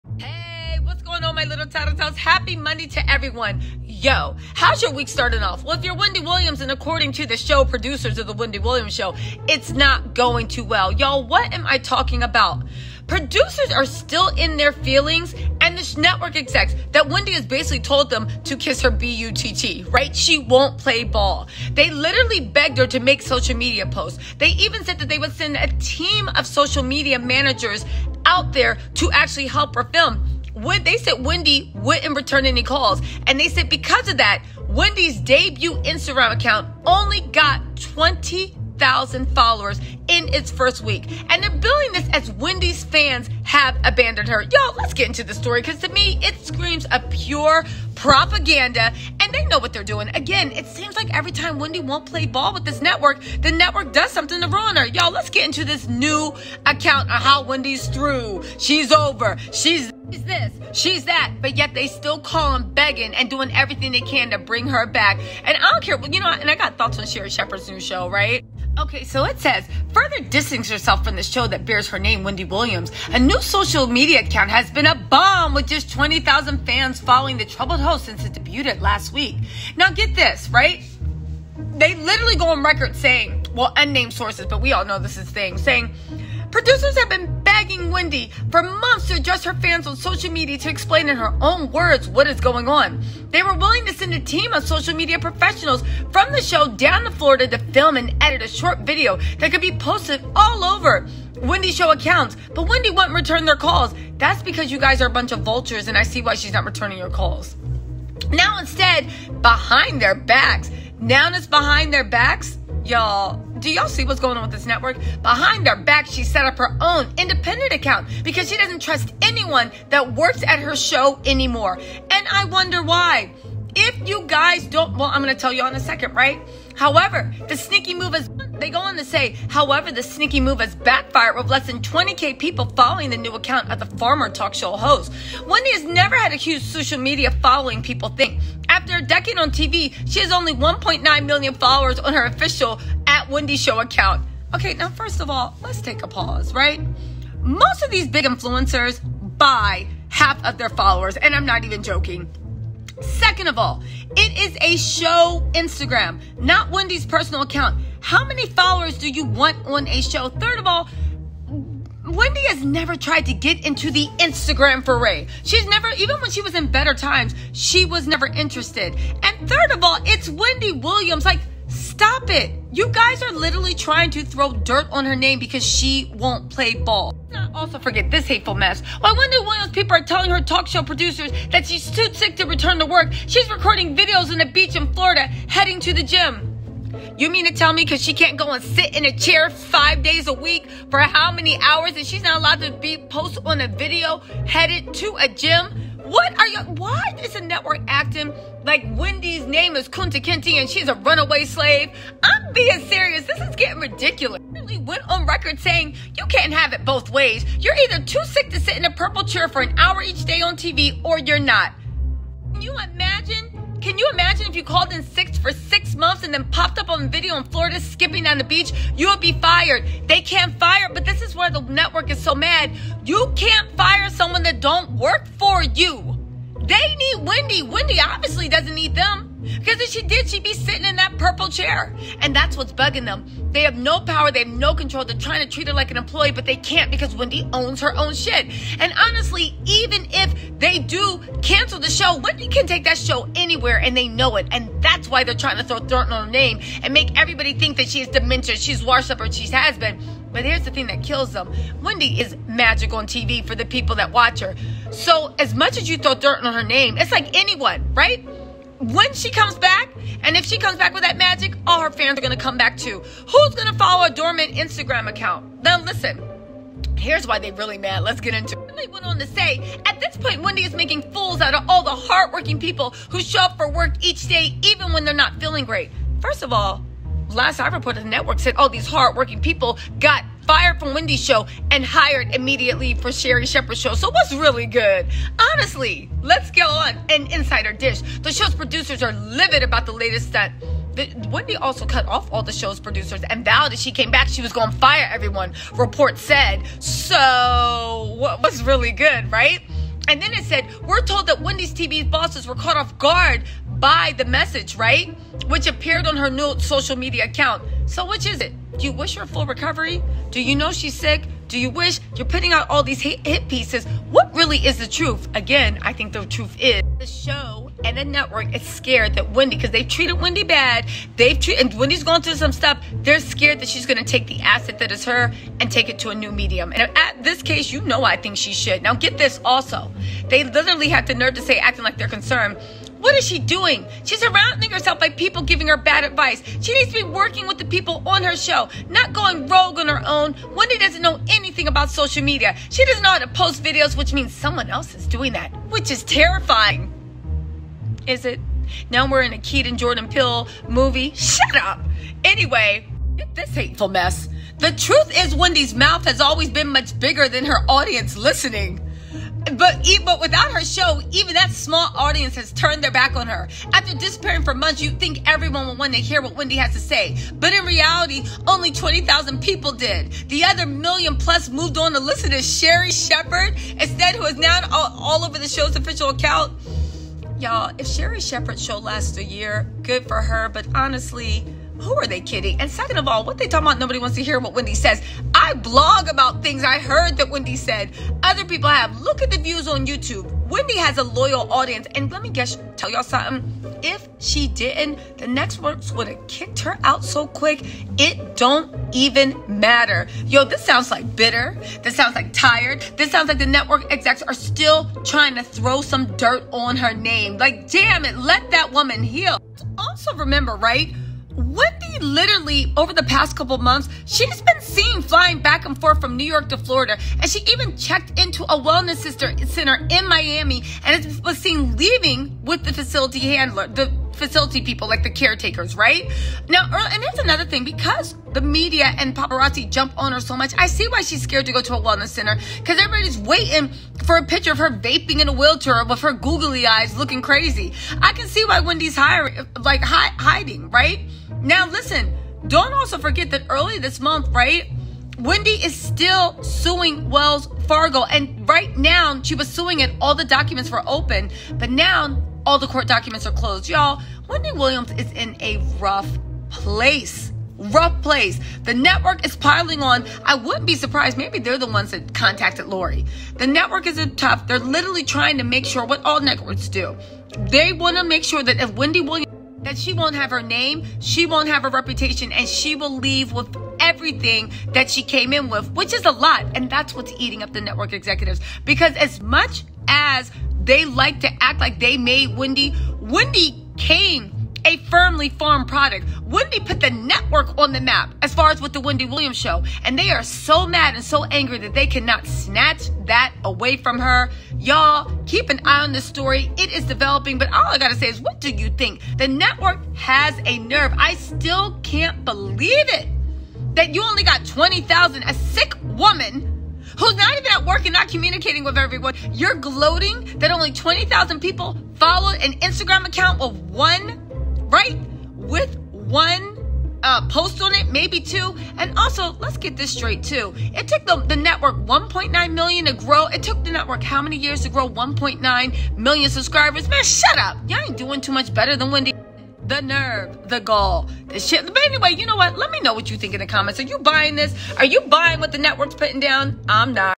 Sources Say Greedy Network Execs Latest Attempt RUIN Wendy’s Reputation Has Gone To Far
Don't forget I also gave a detailed review and SUMMARY of the actual WELLS FARGO vs WENDY WILLIAMS COURT FILINGS to give you guys verifiable receipts!!!  Here in the LINK If You Are Watching This On PlayBack